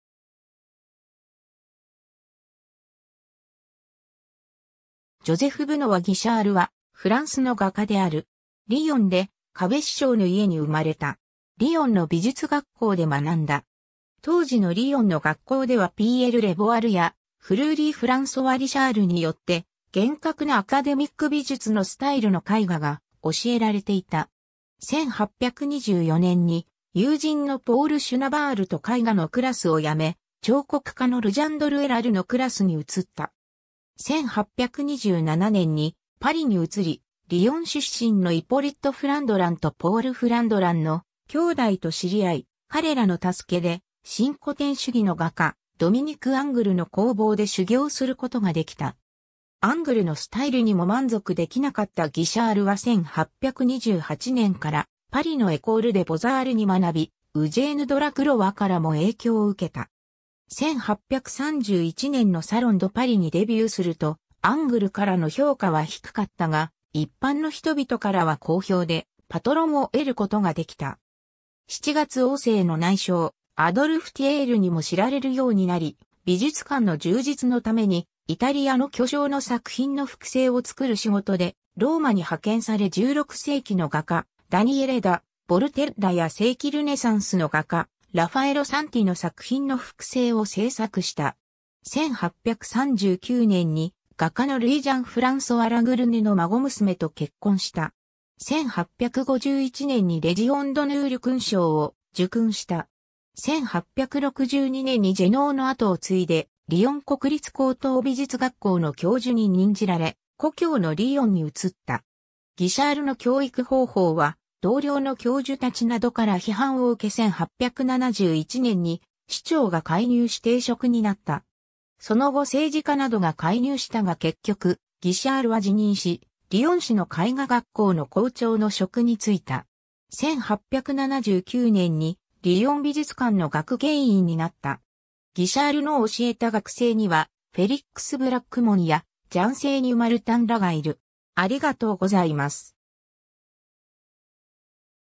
ジョゼフ・ギシャール
ジョゼフ・ブノワ・ギシャール（Joseph Benoît Guichard、1806年11月14日 - 1880年5月31日）はフランスの画家である。
 リヨンで壁紙商の家に生まれた。
リヨンの美術学校で学んだ。
当時のリヨンの学校ではピーエル・レヴォワル(Pierre Révoil)やフルーリ＝フランソワ・リシャール(Fleury-Francois Richard)によって厳格なアカデミック美術のスタイルの絵画が教えられていた。
1824年に友人のポール・シュナヴァールと絵画のクラスを辞め、彫刻家のルジャンドル＝エラル(Jean-François Legendre-Héral)のクラスに移った。
 1827年にパリに移り、リヨン出身のイポリット・フランドランとポール・フランドランの兄弟と知り合い、彼らの助けで、新古典主義の画家、ドミニク・アングルの工房で修行することができた。
アングルのスタイルにも満足できなかったギシャールは1828年からパリのエコール・デ・ボザールに学び、ウジェーヌ・ドラクロワからも影響を受けた。
1831年のサロン・ド・パリにデビューすると、アングルからの評価は低かったが、一般の人々からは好評で、パトロンを得ることができた。
 7月王政の内相、アドルフ・ティエールにも知られるようになり、美術館の充実のためにイタリアの巨匠の作品の複製を作る仕事でローマに派遣され16世紀の画家、ダニエレ・ダ・ヴォルテッラや盛期ルネサンスの画家、ラファエロ・サンティの作品の複製を製作した。
 1839年に画家のルイ＝ジャン＝フランソワ・ラグルネの孫娘と結婚した。
 1851年にレジオンドヌール勲章を受勲した。
1862年にジェノー(Michel Philibert Genod:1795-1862)の後を継いで、リヨン国立高等美術学校の教授に任じられ、故郷のリヨンに移った。
ギシャールの教育方法は同僚の教授たちなどから批判を受け1871年に市長が介入し停職になった。
その後政治家などが介入したが結局、ギシャールは辞任し、リヨン市の絵画学校の校長の職に就いた。
1879年にリヨン美術館の学芸員になった。
ギシャールの教えた学生には、フェリックス・ブラックモンやジャン・セーニュマルタンらがいる。